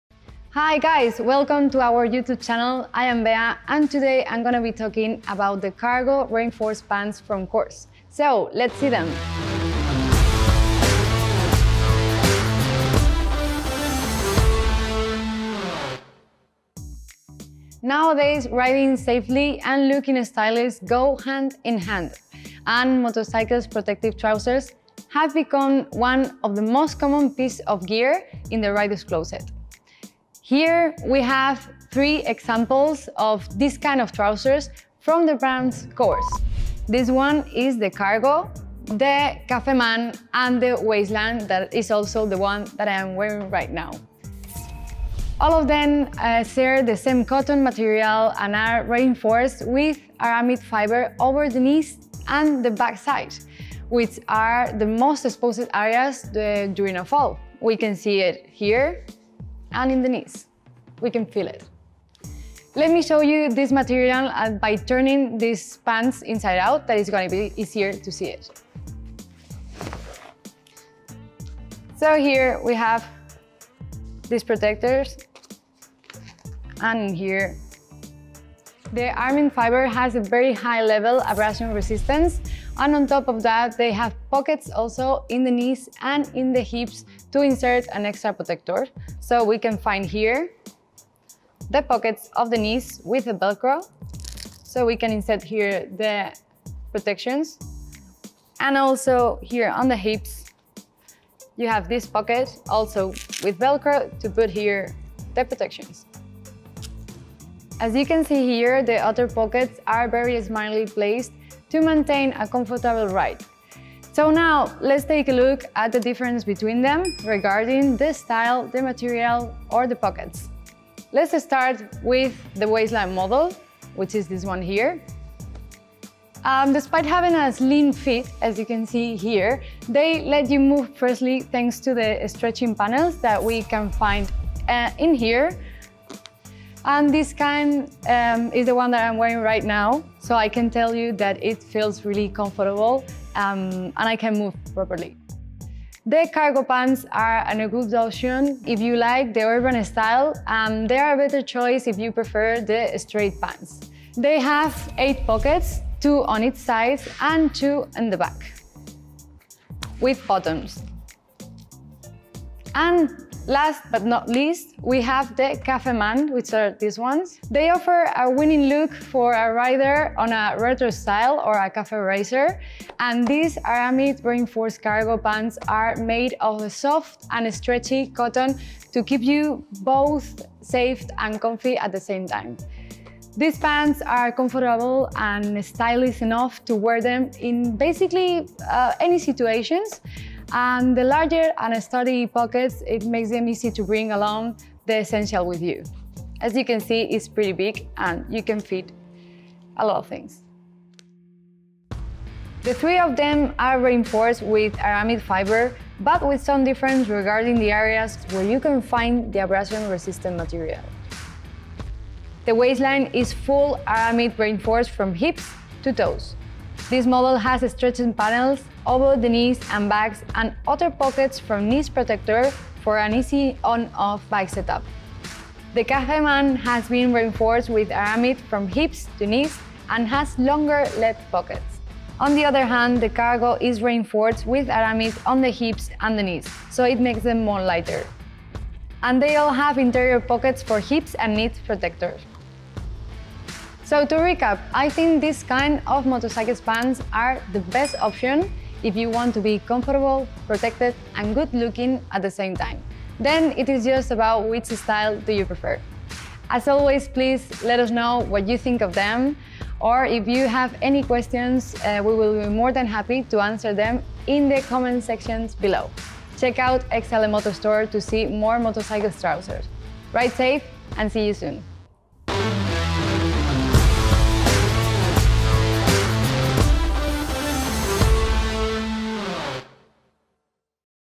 The Best Riding Pants - Course Pants Reviewed And Explained
Motorcycle cargo pants and jeans have revolutionized the riding a lot since they they first hit the market. They are easy to ware and give you a decent level of protection and the best is that they look like regular jeans or trouser. In this video you will see three model of course motorcycle riding pants with different look and style. If you’re interested in riding safe and with casual style  you’ll want to see this video.